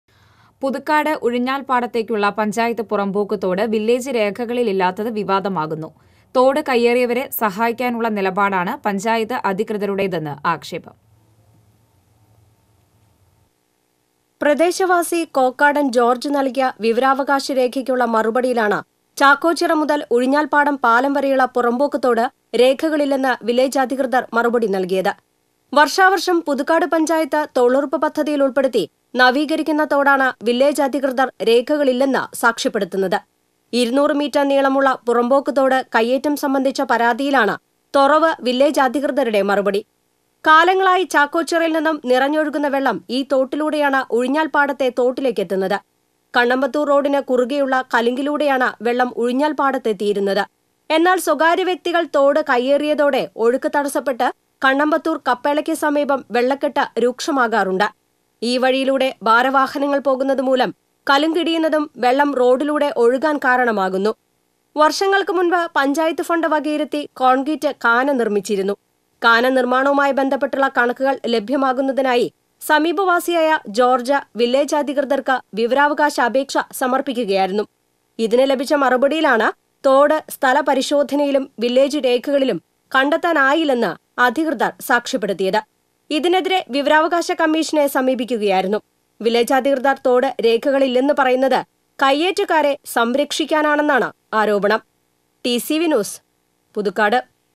കയ്യേറ്റക്കാരെ സഹായിക്കാന്‍ പഞ്ചായത്ത് അധികൃതര്‍ | TCV Pudukkad
പുതുക്കാട് ഉഴിഞ്ഞാല്‍ പാടത്തേക്കുള്ള പഞ്ചായത്ത് പുറമ്പോക്ക് തോട് വില്ലേജ് രേഖകളില്‍ ഇല്ലാത്തത് വിവാദമാകുന്നു.   തോട് കൈയ്യേറിയവരെ സഹായിക്കാനുള്ള നിലപാടാണ് പഞ്ചായത്ത് അധികൃതരുടേതെന്ന് ആക്ഷേപം